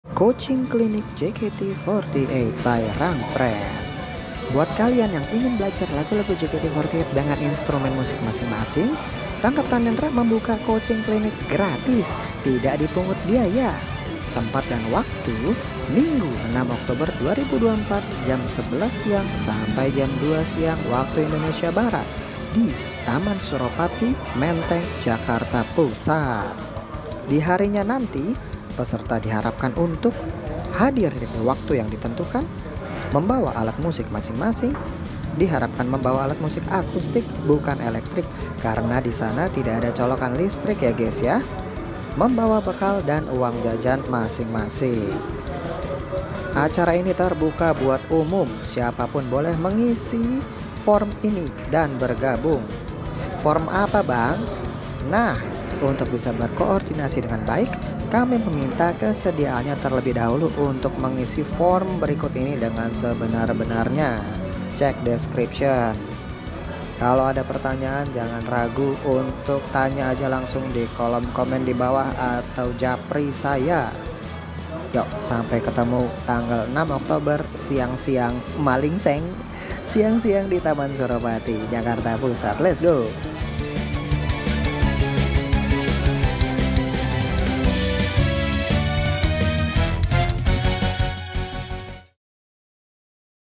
Yuk Ikutan COACHING CLINIC JKT48 by Rangpren!!
Buat kalian yang ingin belajar lagu-lagu JKT48 dengan instrumen musik masing-masing, Rangga Pranendra membuka coaching clinic gratis (tidak dipungut biaya). Tempat dan waktu:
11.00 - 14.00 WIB
Taman Suropati, Menteng, Jakarta Pusat

TERBUKA UNTUK UMUM! Siapa pun boleh mengisi form ini dan bergabung.

Di harinya nanti, peserta diharapkan untuk
1. hadir di waktu yang ditentukan,
2. membawa alat musik masing-masing. Diharapkan membawa alat musik akustik (bukan elektrik) karena di sana tidak ada colokan listrik ya gaes ya,
3. membawa bekal dan uang jajan masing-masing.

Untuk bisa berkoordinasi dengan baik, kami meminta kesediaannya terlebih dahulu untuk mengisi form berikut ini dengan sebenar-benarnya.

SAMPAI JUMPA!